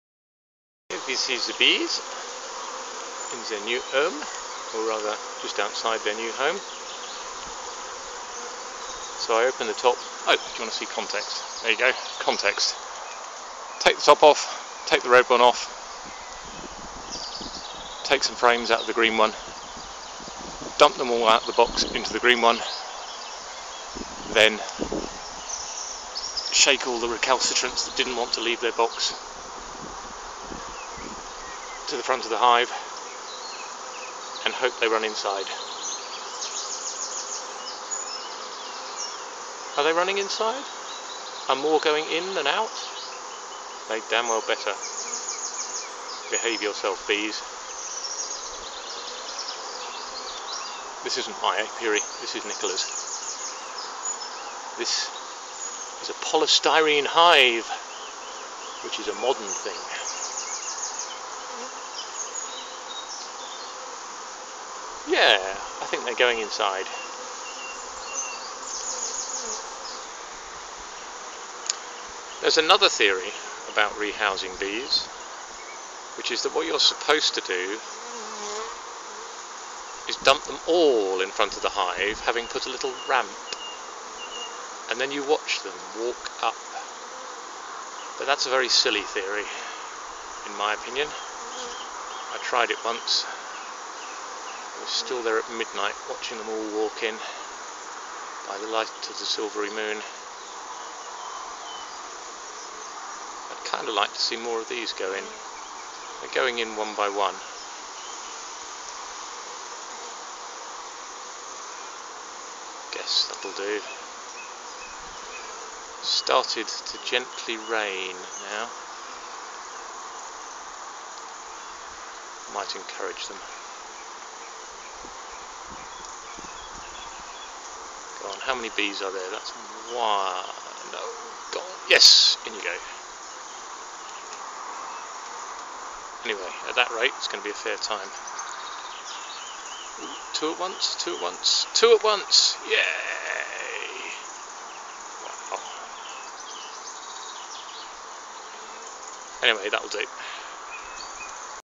Madingley bees
These are the bees I picked up from Madingley Hall and left with Nikola in the back of her garden in Nicholas's polystyrene hive.

Most of them went into the hive (open it up, remove half the frames, shake the box in, put the frames back, close up the hive) and then the rest got shaken in front of the hive, where they will gradually walk in. I trust.